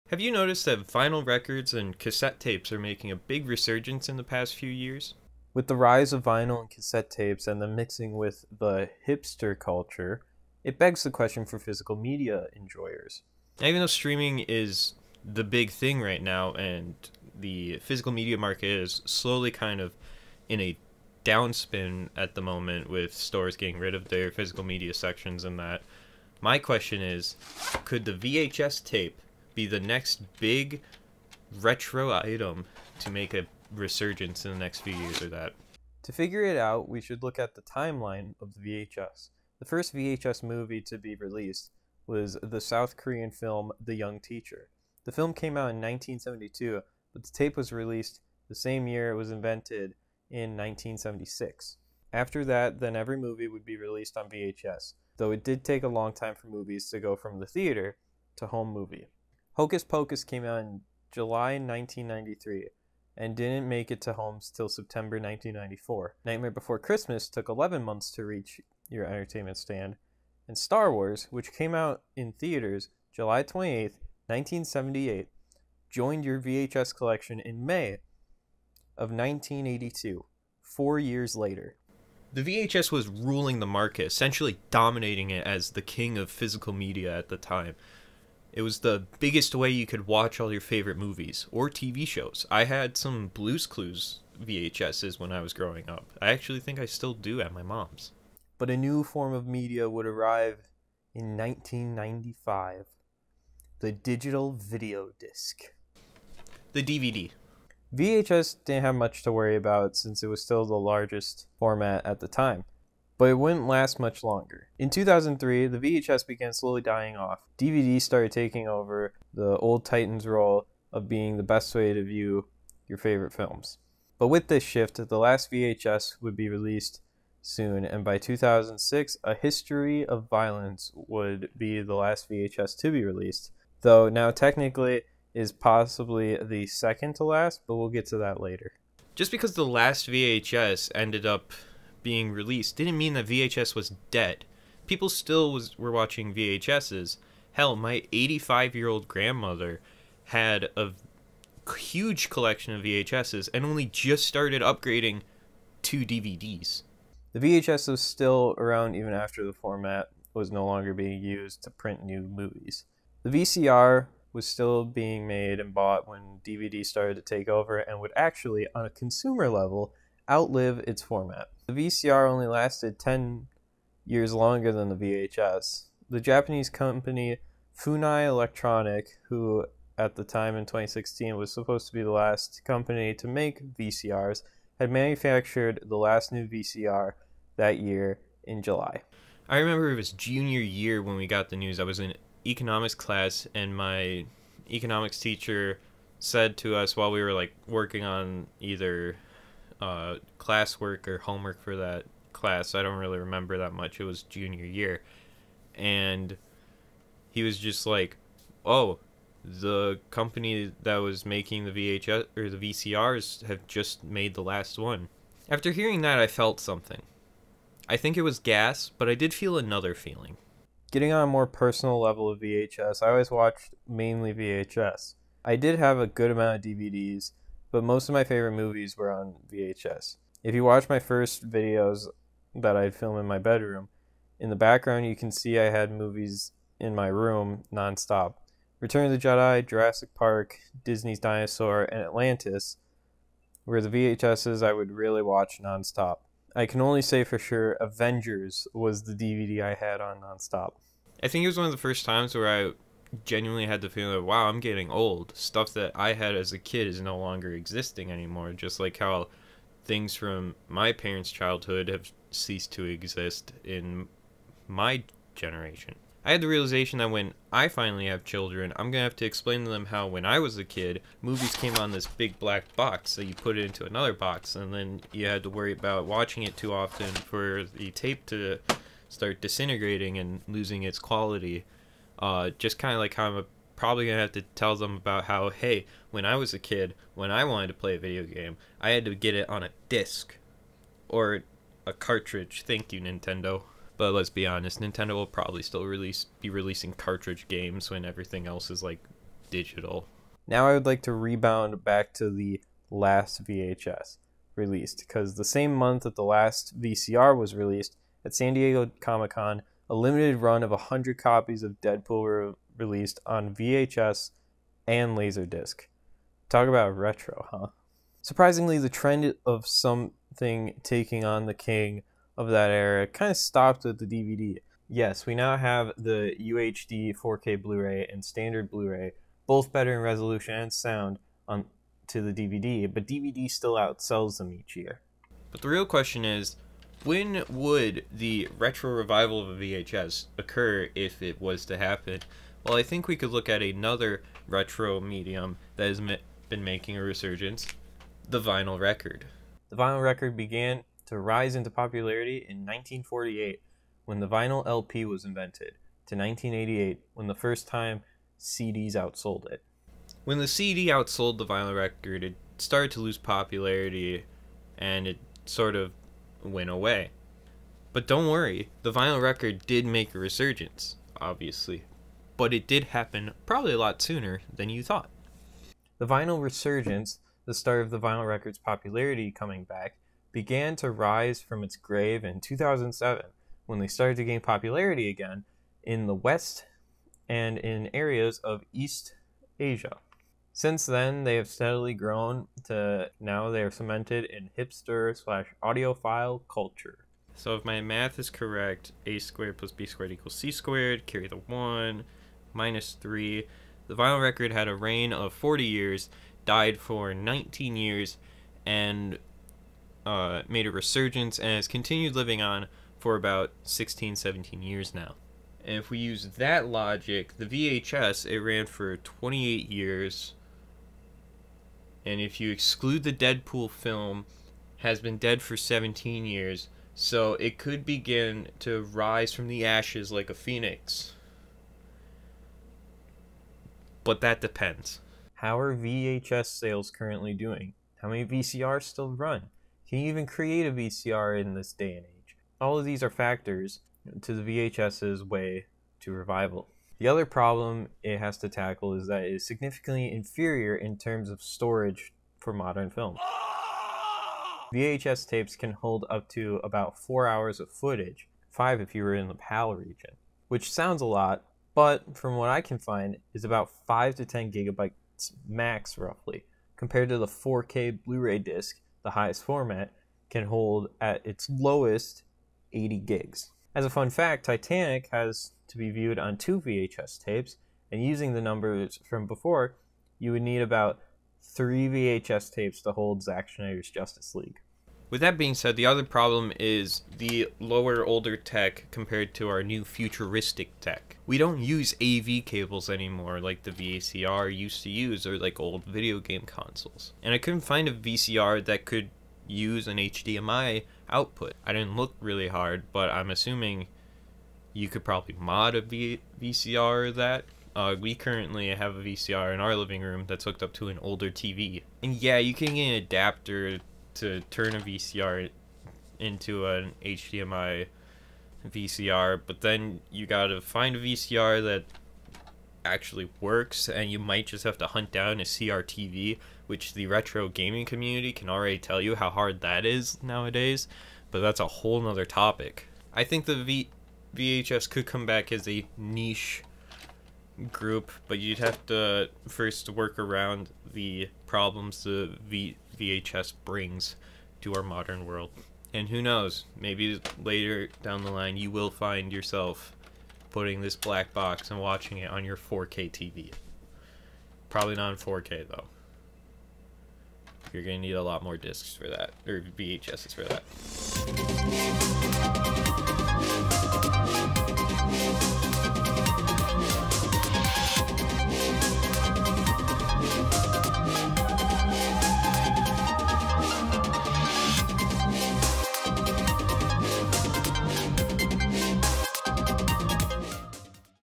Could VHS Make A Comeback?
Today, we look at the VHS and ask ourselves with vinyl and cassetes making a comeback... could the VHS Tape make a comeback?

Articles:
Why did films take so long to be released to home video

I will see yooooooou in the future
bye guys